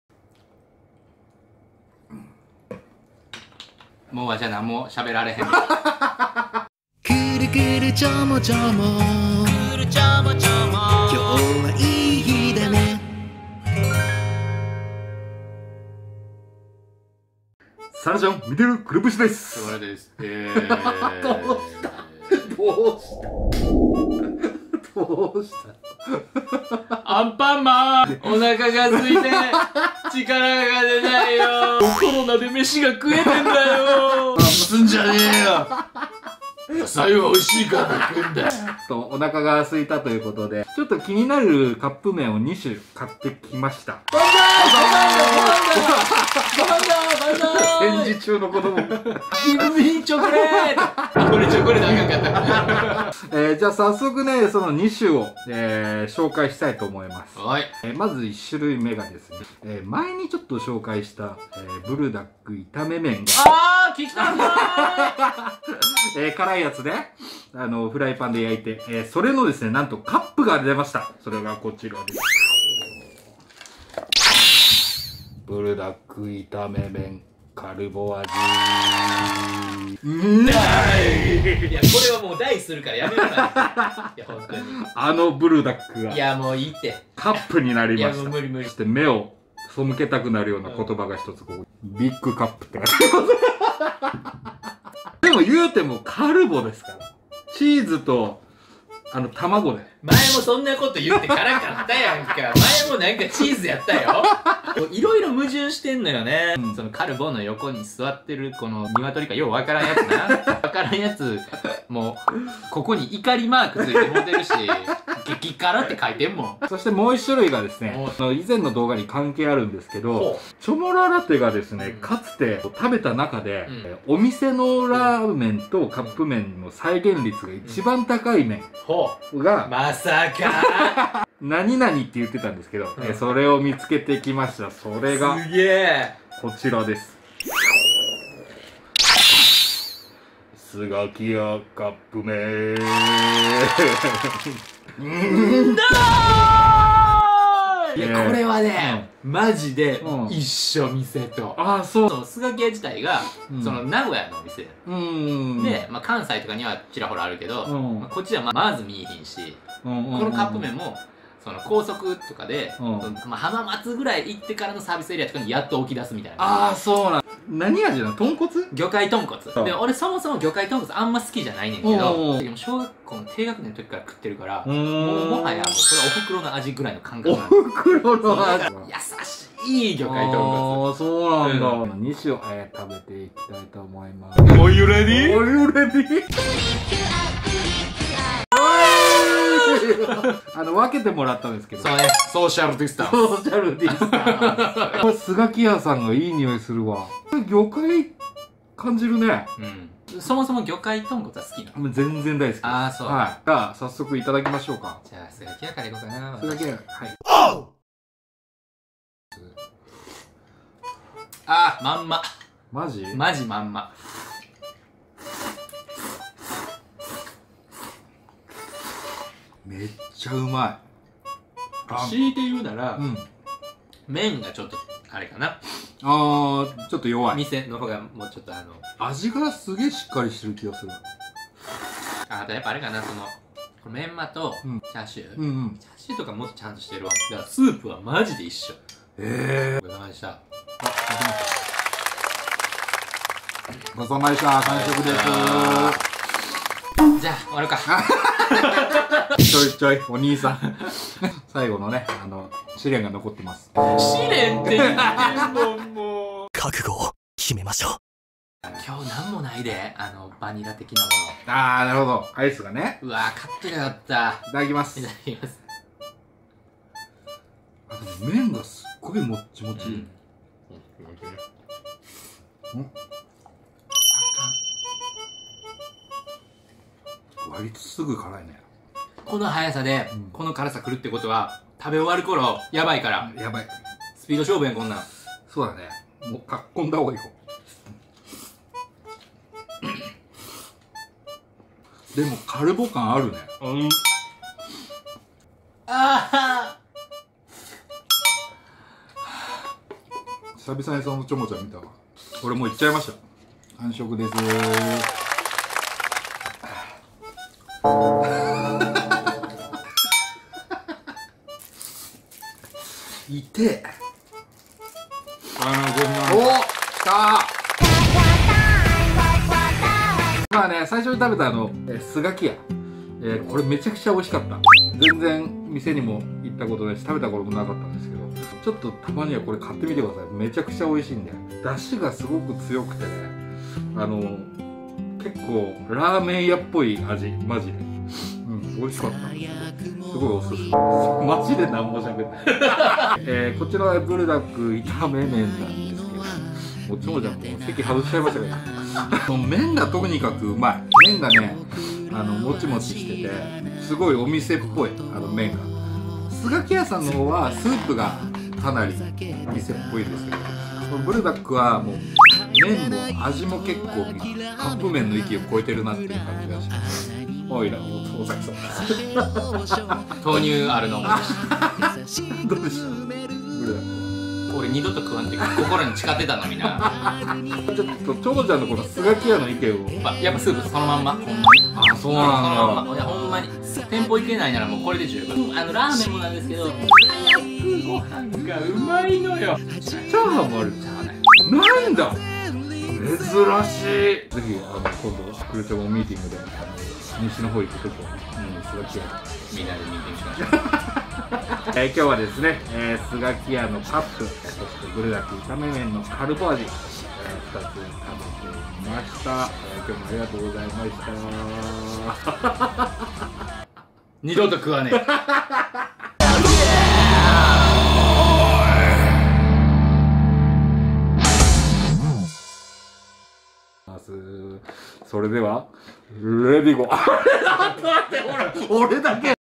気になるカップ麺２種類買って食べてみた【伏線回収】
クルチョモchannelでも様々な麺を食べて来ましたが、
過去動画で触れた、
「お店のラーメンを忠実に再現されてる」とチョモが語った
あのカップ麺と

これまた過去に食べた激辛の袋麺がカップになった、
あのシリーズを

運よく見つけて来たので
一緒にレビュー！！

美味いのかアウトなのか、
クルチョモがガチ判定！！

さぁ、みんなで不摂生しましょう！！

 [過去動画リンク]

お店味のコンビニ麺２種、どのくらい再現されてるか食べてみた